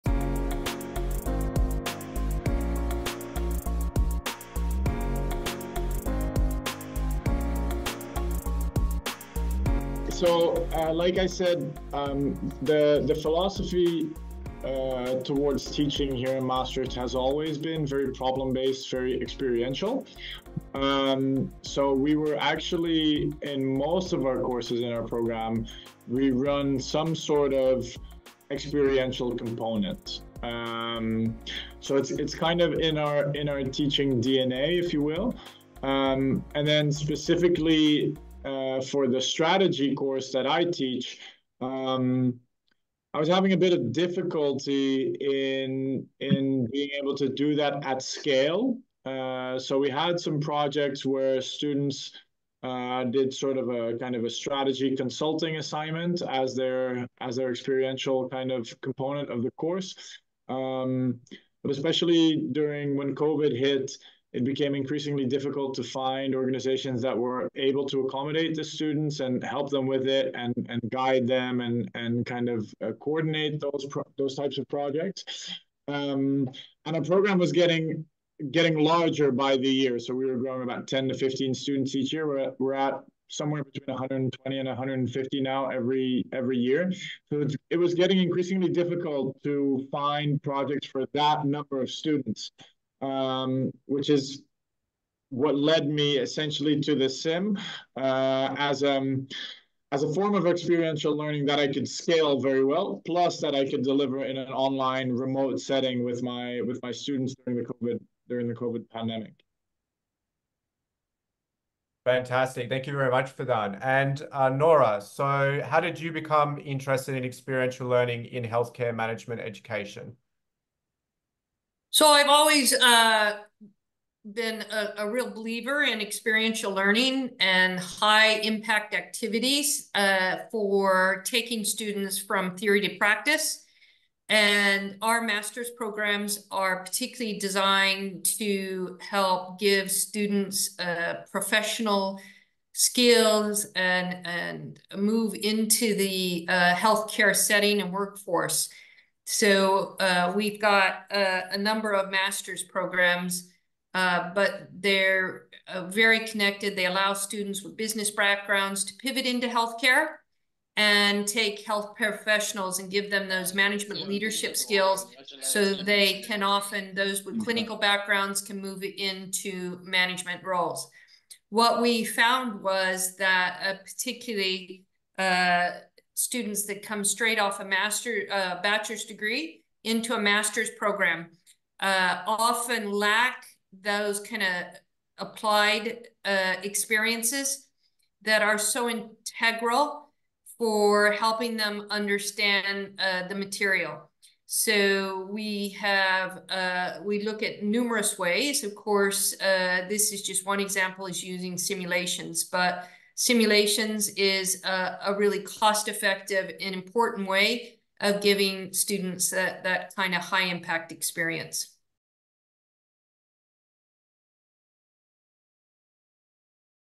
Business Simulations for Executive Training - Question 1
How did you become interested in experiential learning in healthcare management education?